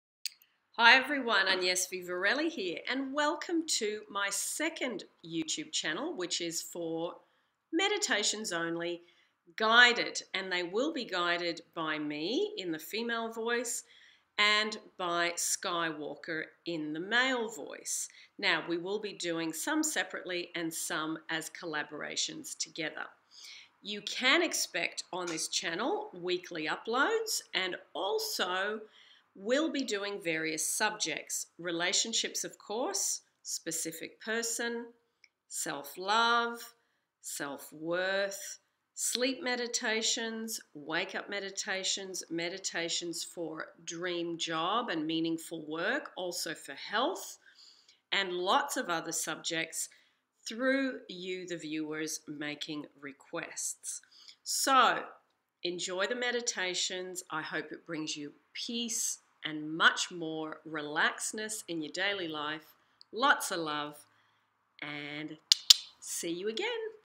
Guided Meditation Channel Trailer
In this YouTube Agnes announces what you can expect to hear on this  Guided Meditations with Music Channel. Each week approx Mediations will be uploaded on the subjects of Relationships, Specific Person, Dream Job and Meaningful Work as well as for Sleep, Relaxation, Self Love, Improving Health, Desired Travel, Dream Home and changing your Mindset to attract these things into your life. They will be in my voice and also in Skywalkers male voice. Some will be done individually and some will be collaborations.